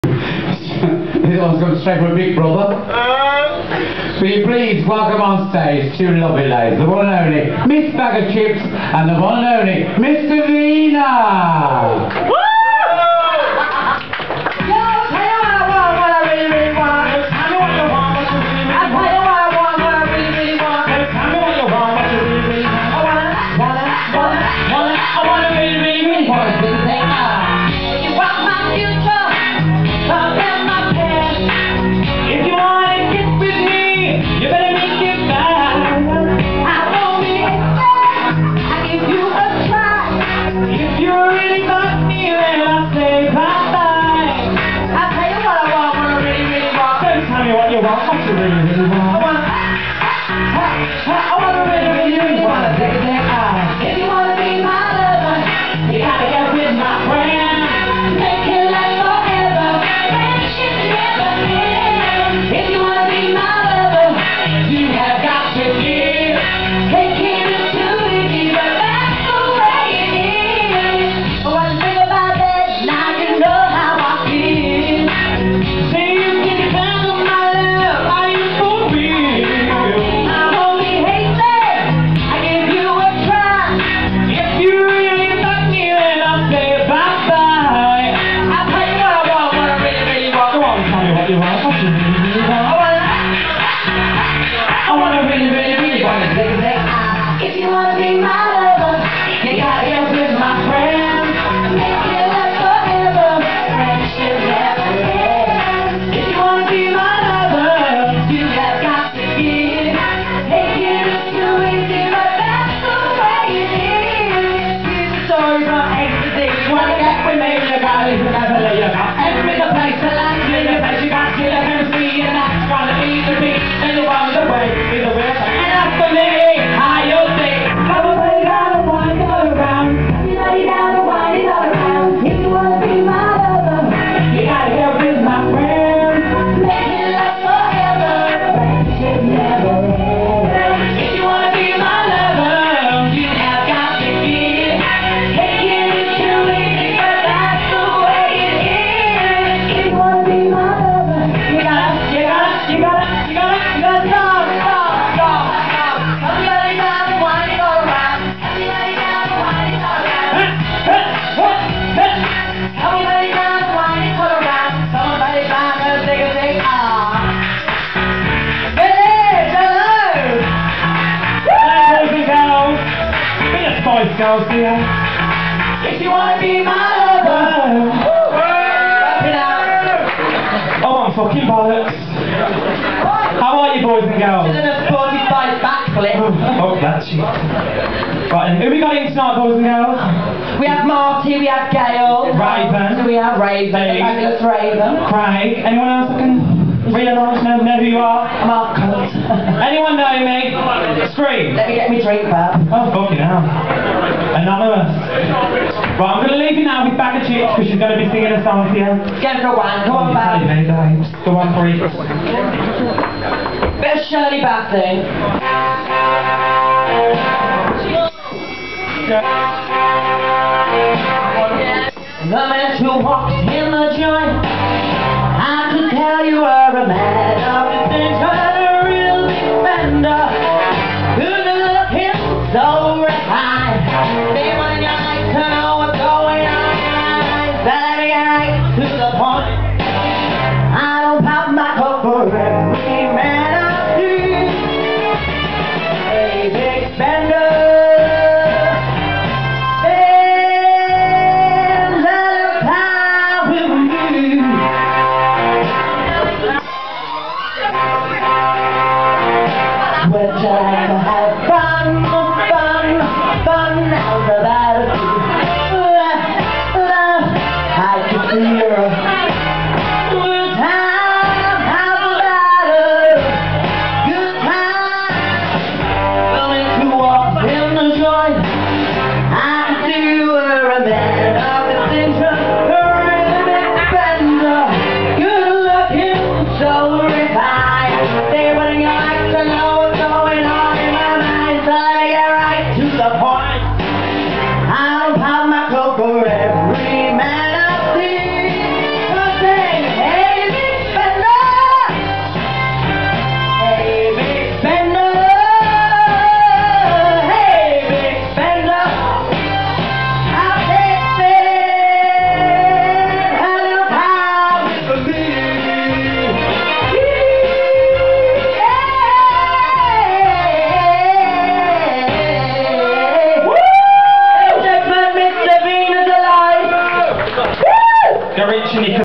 Baga Chipz and Davina singing
Baga Chipz and Davina sing Spice Girls, then Baga Chipz sings Shirley Bassey.